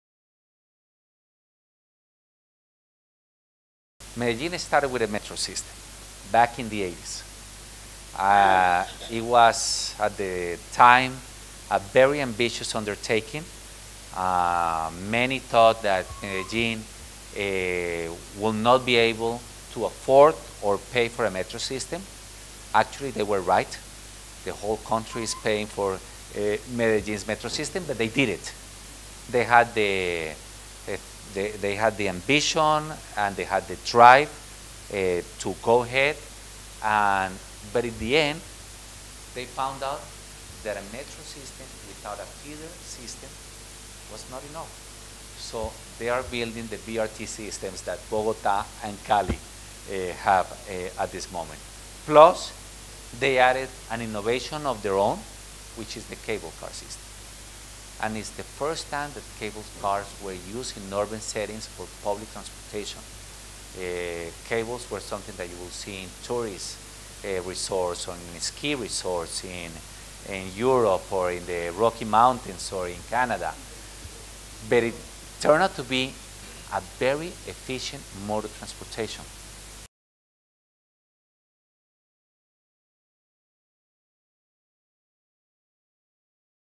Santiago Castro-Gomez: Public transport with cable cars
Santiago Castro-Gomez, a former congressman and former director-general of the Colombia Civil Aviation Authority, shared transformations of three Colombian cities — Bogotá, Medellín and Cali — in a CLC Lecture.